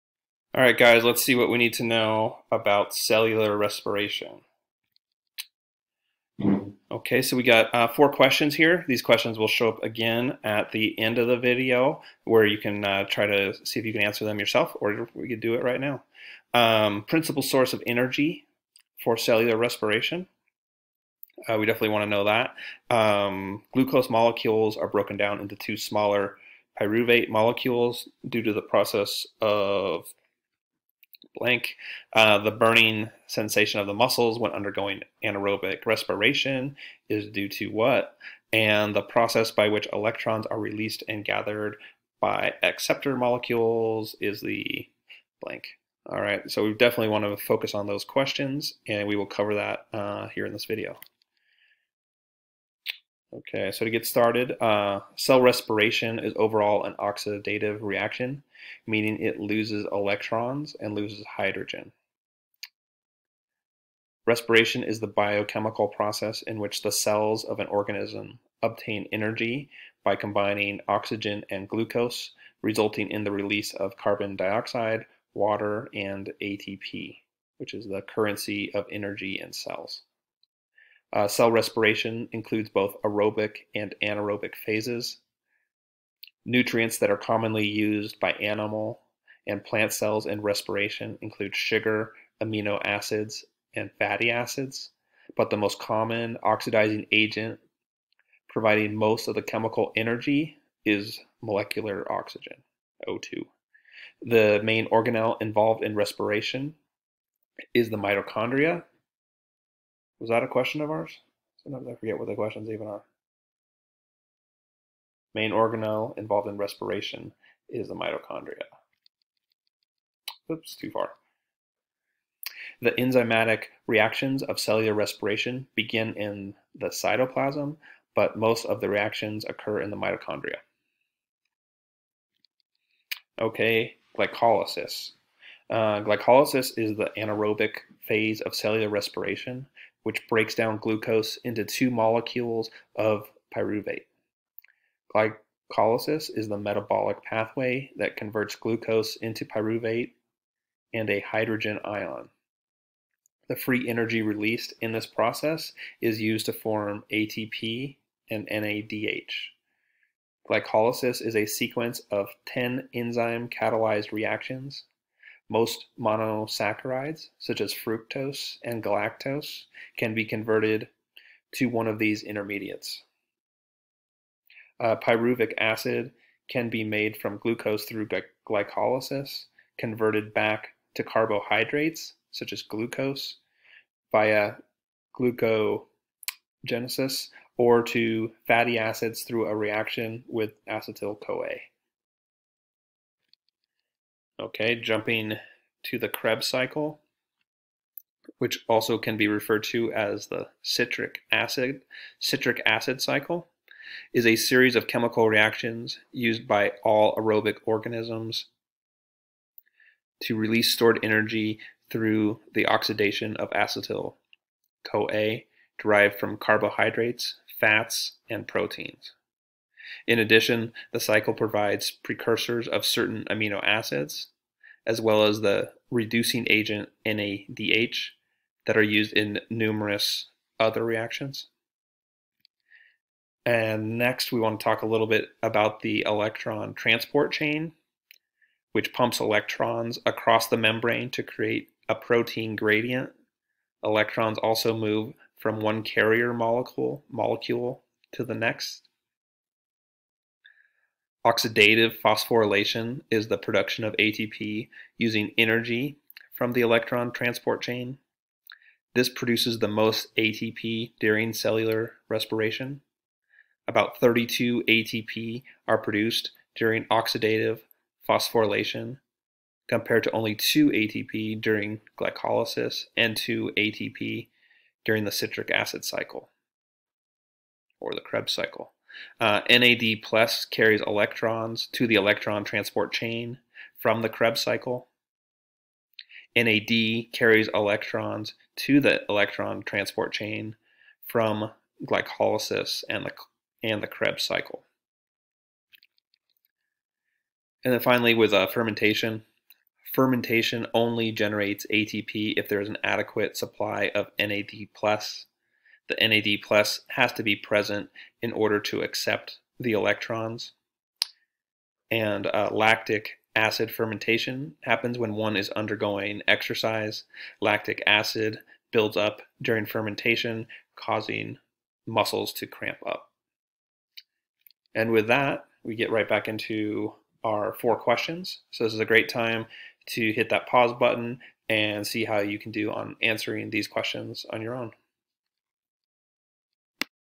HESI A2 Exam: Cellular Respiration
• What is the principal source of energy for cellular respiration?

• Glucose molecules are broken down into two smaller pyruvate molecules due to the process of _________

• The burning sensation of the muscles when undergoing anaerobic respiration is due to the __________

• The process by which electrons are released and gathered by acceptor molecules is the __________

Intro

Cell respiration is overall an oxidative reaction, meaning it loses electrons and loses hydrogen.

Respiration is the biochemical process in which the cells of an organism obtain energy by combining oxygen and glucose, resulting in the release of carbon dioxide, water, and ATP (the currency of energy in cells).

Cell respiration includes both aerobic and anaerobic phases.

Nutrients that are commonly used by animal and plant cells in respiration include sugar, amino acids and fatty acids, but the most common oxidizing agent providing most of the chemical energy is molecular oxygen (O2).

The main organelle involved in respiration is the mitochondria. The enzymatic reactions of cellular respiration begin in the cytoplasm, but most of the reactions occur in the mitochondria.

Glycolysis
Glycolysis is the anaerobic phase of cellular respiration which breaks down glucose into two molecules of pyruvate. Glycolysis is the metabolic pathway that converts glucose into pyruvate and a hydrogen ion. The free energy released in this process is used to form ATP and NADH. Glycolysis is a sequence of ten enzyme-catalyzed reactions. Most monosaccharides, such as fructose and galactose, can be converted to one of these intermediates.

Pyruvic acid can be made from glucose through glycolysis, converted back to carbohydrates (such as glucose) via gluconeogenesis, or to fatty acids through a reaction with acetyl-CoA.

Krebs Cycle
The citric acid cycle (CAC) – also known as the Krebs cycle – is a series of chemical reactions used by all aerobic organisms to release stored energy through the oxidation of acetyl-CoA derived from carbohydrates, fats, and proteins. In addition, the cycle provides precursors of certain amino acids, as well as the reducing agent NADH, that are used in numerous other reactions.

Electron Transport Chain

The electron transport chain pumps electrons across the membrane to create a proton gradient. Electrons also move from one carrier molecule to the next.

Oxidative phosphorylation is the production of ATP using energy from the electron transport chain. This produces the most ATP during cellular respiration. About 32 ATP are produced during oxidative phosphorylation, compared to 2 ATP during glycolysis and 2 ATP during the citric acid cycle.

NAD+ carries electrons to the electron transport chain from the Krebs Cycle.

NAD carries electrons to the ETC from glycolysis and Krebs cycle.

Fermentation

Fermentation only generates ATP if there is an adequate supply of NAD+. The NAD+ has to be present in order to accept the electrons.

Lactic acid fermentation happens when one is undergoing exercise. Lactic acid builds up during fermentation, causing muscles to cramp up.